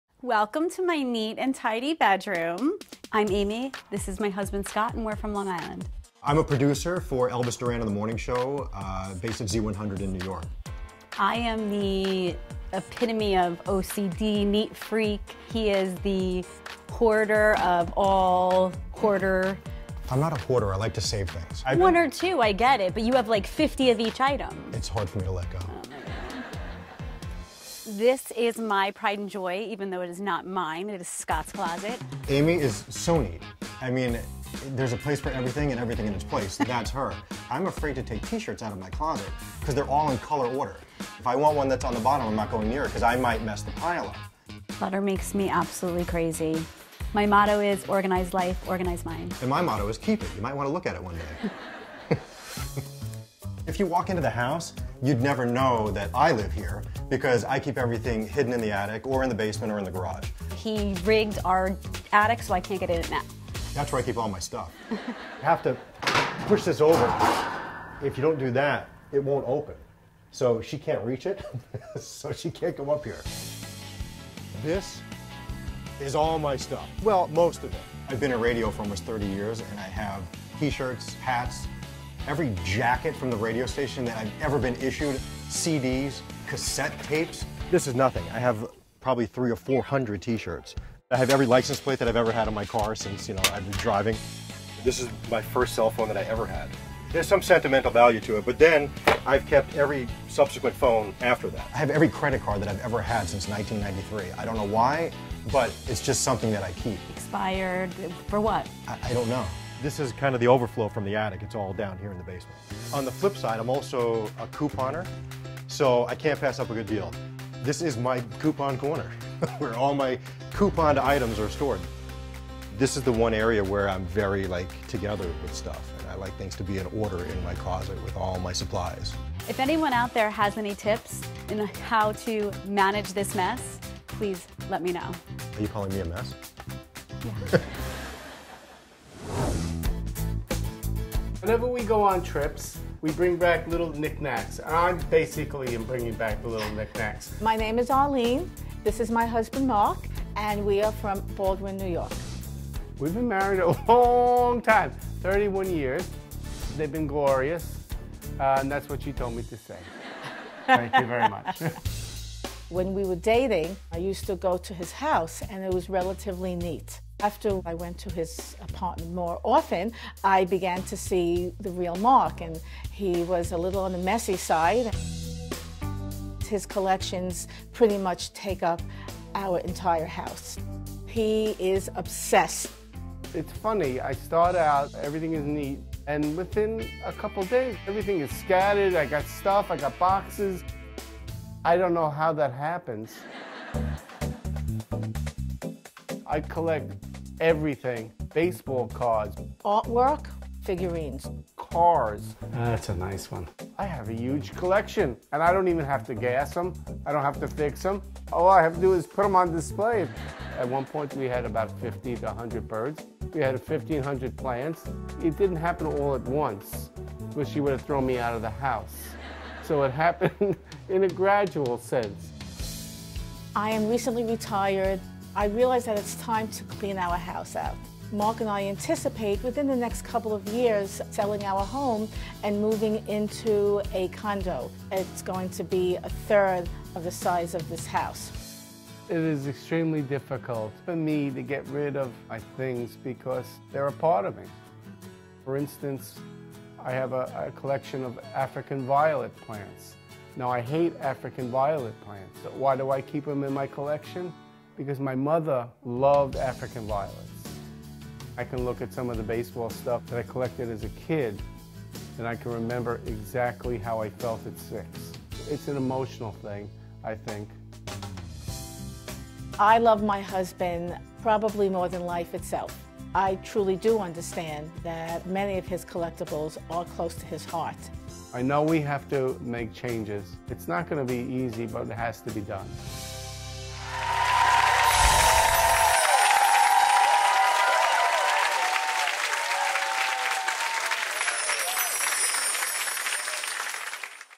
2 Married Couples Share Their Clutter Incompatibility Stories
A "Squeaky Clean vs. Clutter Unseen" couple and a "Tidier vs. Collector" couple share their stories with organizing expert Peter Walsh.